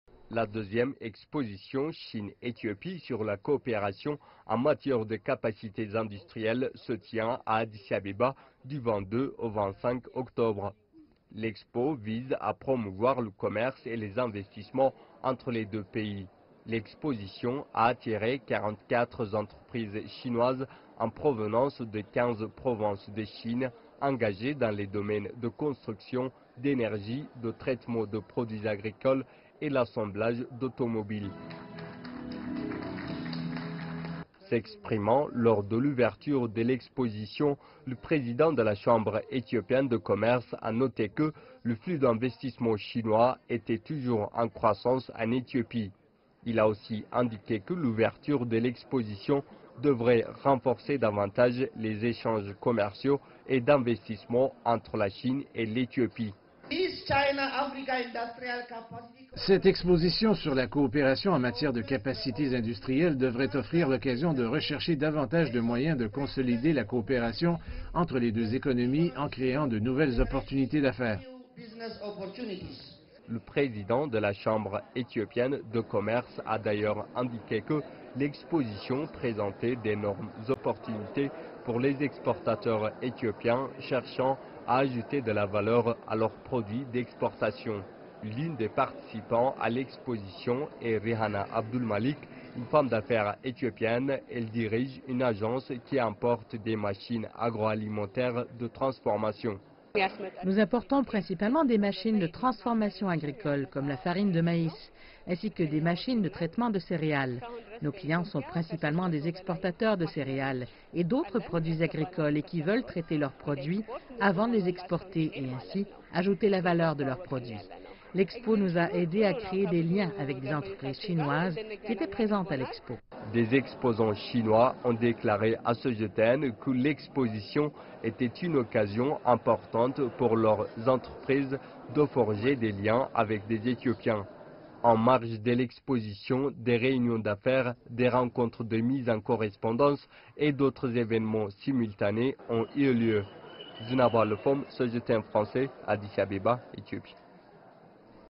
Des entreprises chinoises et éthiopiennes forgent des partenariats plus étroits
La deuxième édition de l’Exposition Chine-Éthiopie sur la coopération en matière de capacités industrielles a eu lieu à Addis-Abeba.
L'Expo, visant à promouvoir la coopération entre les deux pays dans le domaine du développement industriel, a attiré une quarantaine d’entreprises chinoises provenant de 15 provinces du pays.